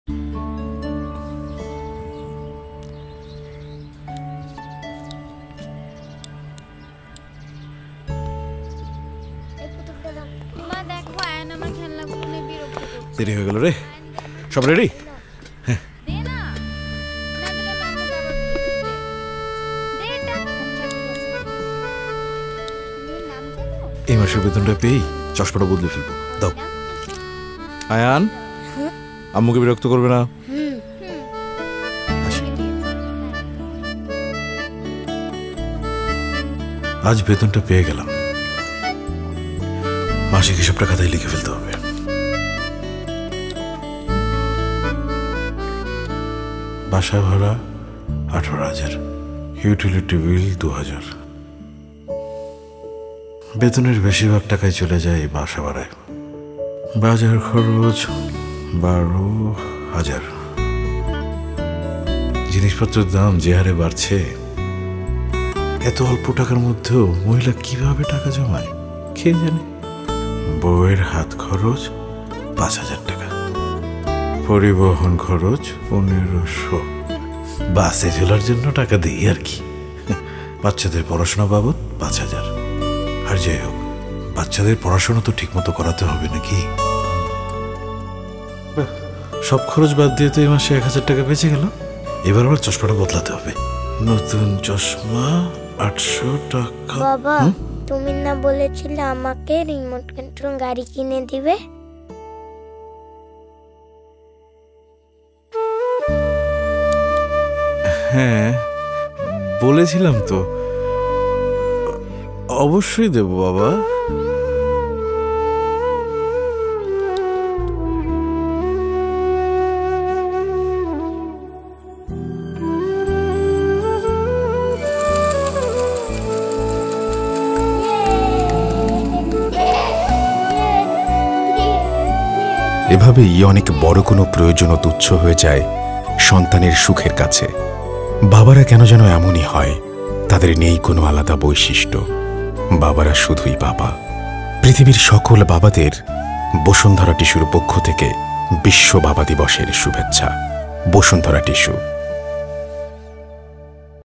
পৃথিবীর সকল বাবাদের বসুন্ধরা টিস্যুর পক্ষ থেকে বিশ্ব বাবা দিবসের শুভেচ্ছা
বাবাদের অনেক বড় কোন প্রয়োজনও তুচ্ছ হয়ে যায় সন্তানের সুখের কাছে।
বাবারা কেন যেন এমনি হয় !
তাদের নেই কোন আলাদা বৈশিষ্ট্য, বাবারা শুধুই বাবা।